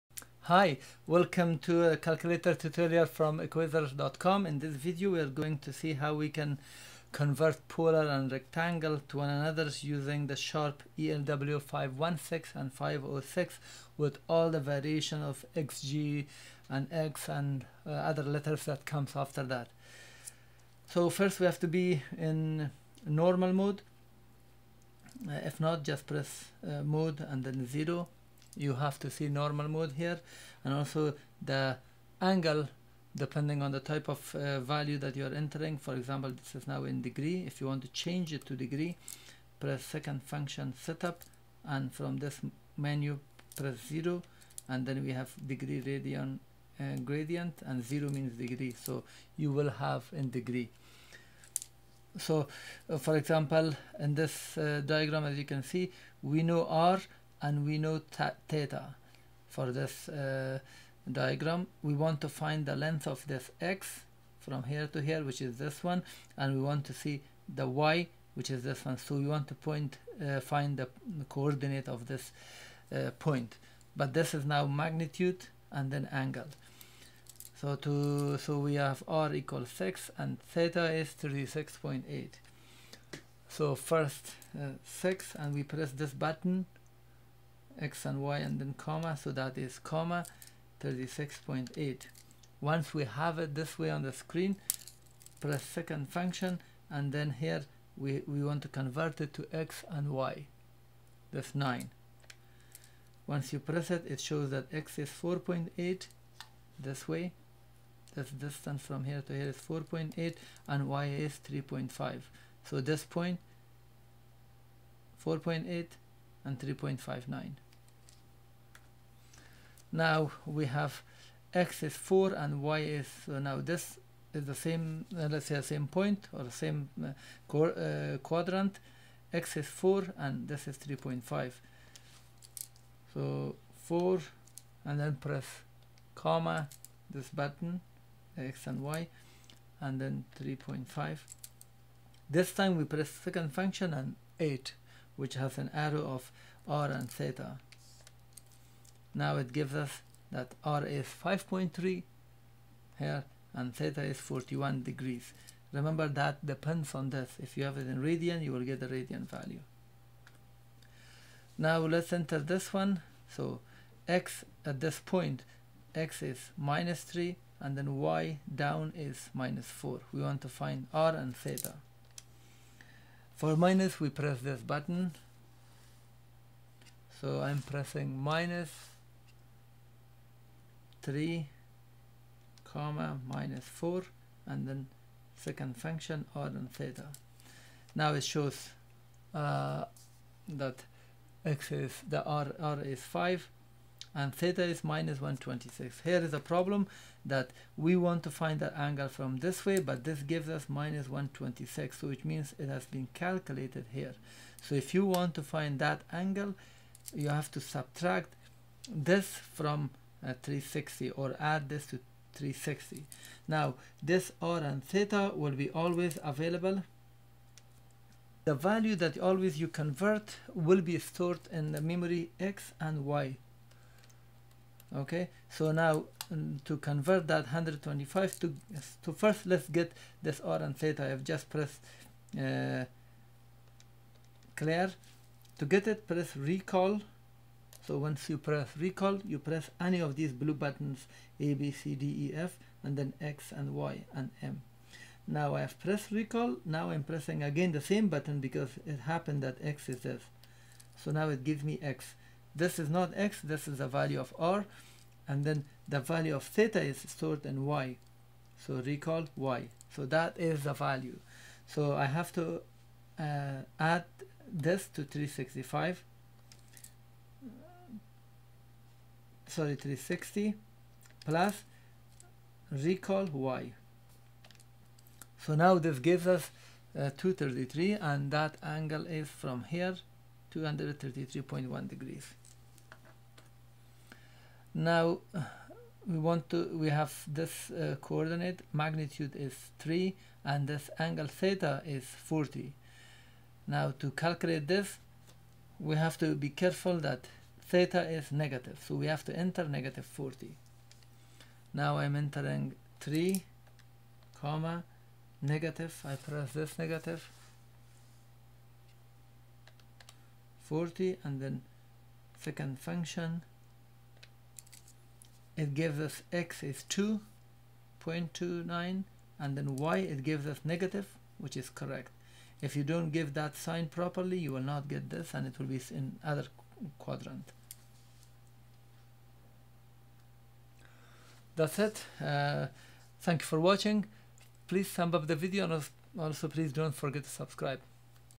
How to convert polar and rectangular coordinates using EL-W516X EL-W506 calcualtor
In this video you will learn how to convert Polar and Rectangular coordinates one to the other using Sharp calculators:
EL-W516XG
EL-W506T
EL-W516T
EL-W516TBSL
EL W516TBSL
EL-W506X
EL-W516X
EL-W506

Tutorial by Equaser